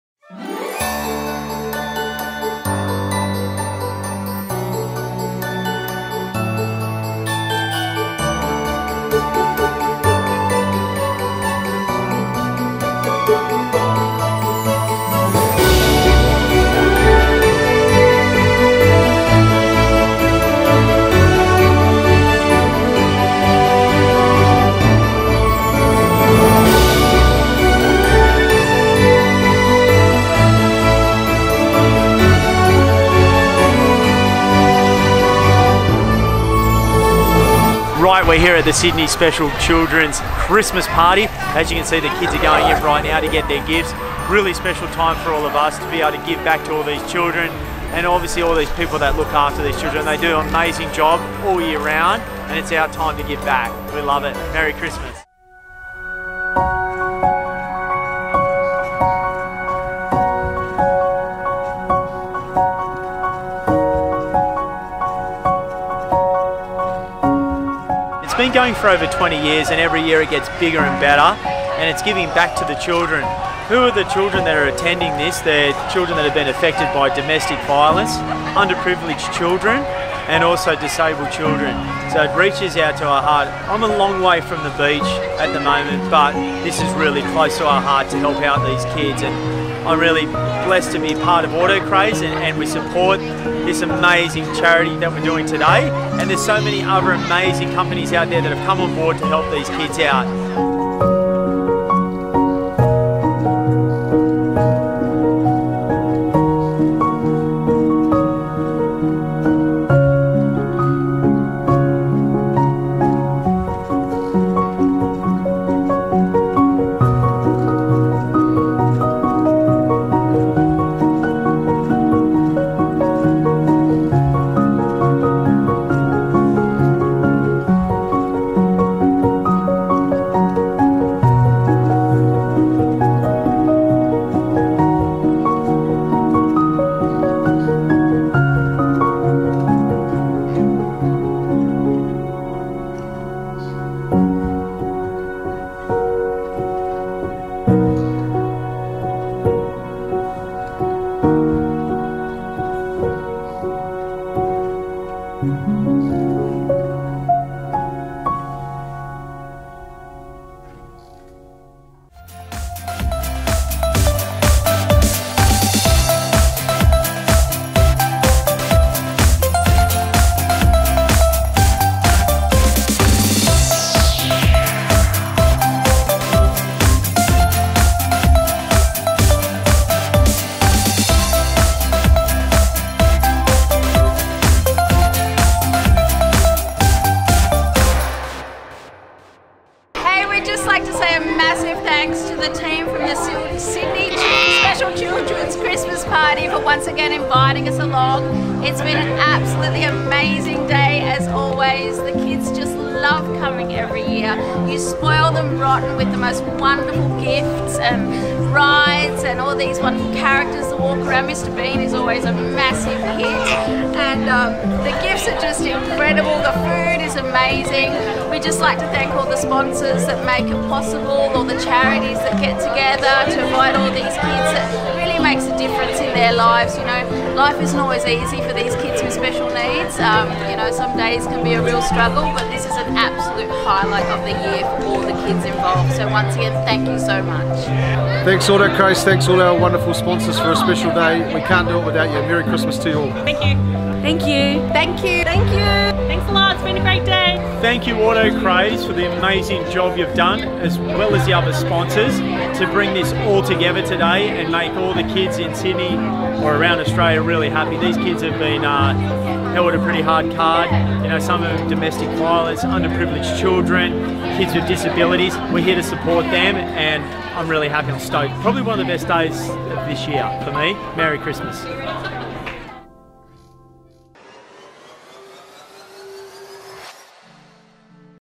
Sydney Special Children's Christmas Party | Sponsored by AutoCraze 2018
We’re never short of experiences at AutoCraze, which also makes us never short of words. But when it came time to write up about this wonderful experience, it’s with a large ball in our throats that we skirmish to find words to share.

Nothing wraps up a year of hard work, blood, sweat and tears like being overwhelmed with gratitude and then shedding real tears for the less fortunate.

We are unbelievably in debt to Special Children’s Christmas Party for allowing us yet again to be a part of their wonderful day. It makes the whole year worthwhile and really puts a perspective on what is important.

We hope your Christmas is filled with Joy and happiness, please remind your loved ones that you love them, make amends and stay safe this season.

With love,

Team AutoCraze.

Remember to follow & like their page: Sydney Special Children's Christmas Parties